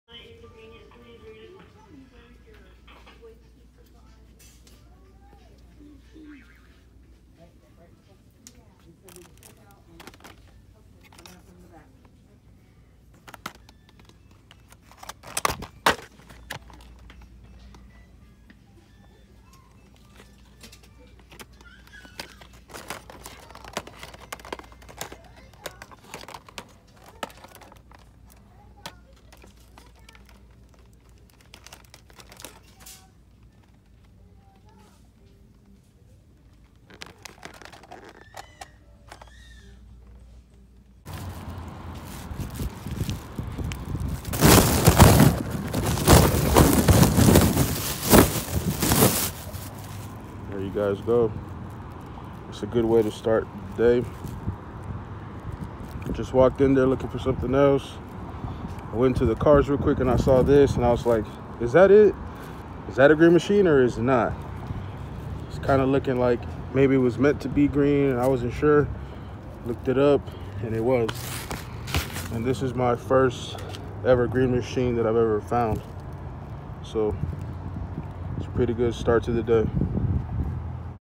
GREENLIGHT: 2019 Shelby Green Machine
I scored a quick greenie! What do you guys think, cool?